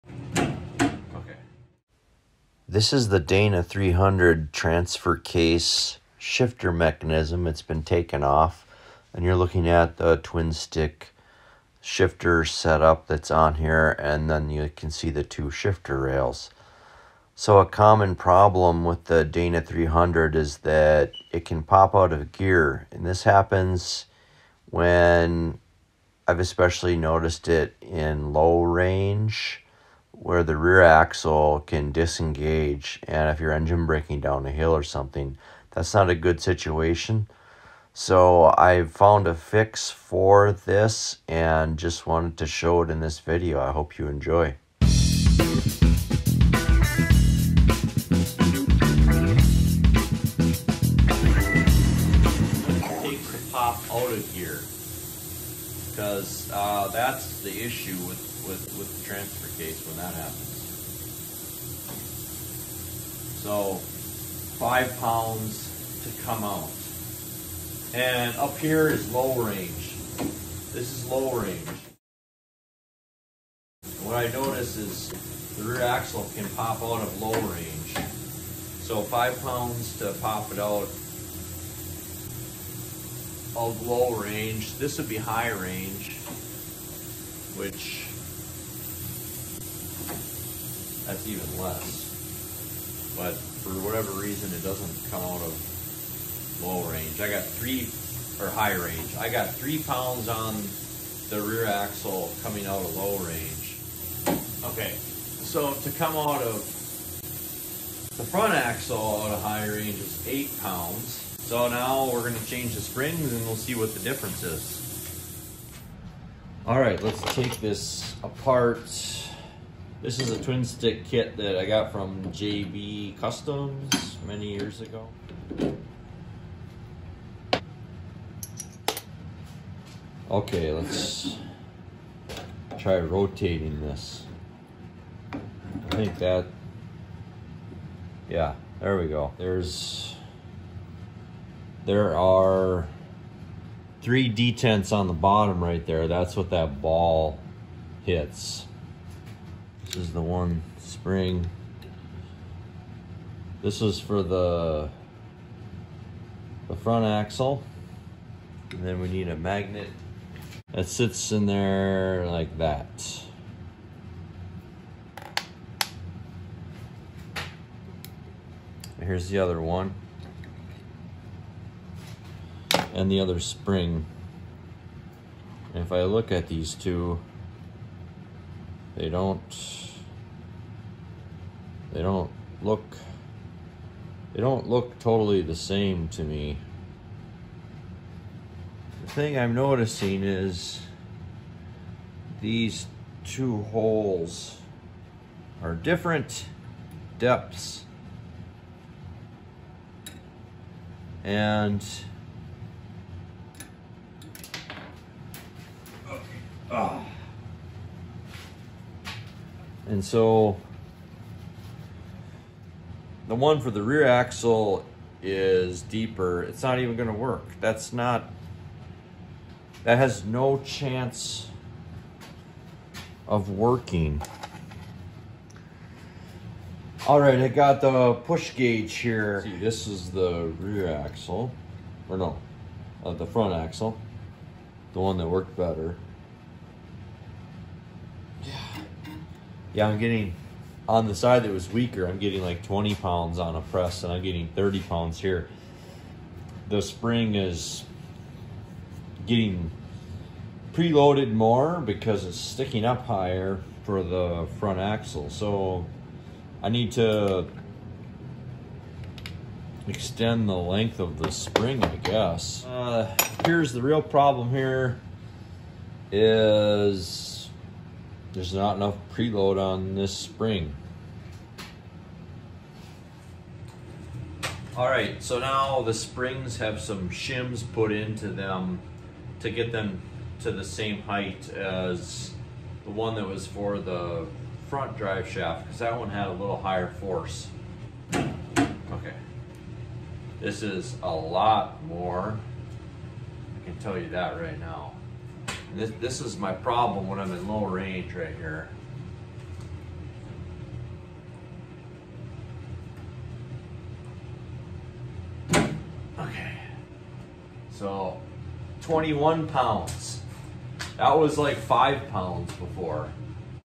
Jeep Dana 300 Transfer Case Shifter Rail Fix
The Jeep Dana 300 transfer case is the undisputed best stock transfer case that ever came in the Jeep CJ’s.  It does have a problem of popping out of gear under load at times.  In this video I made the shifter rail require a lot more force to move which should keep it in gear much longer.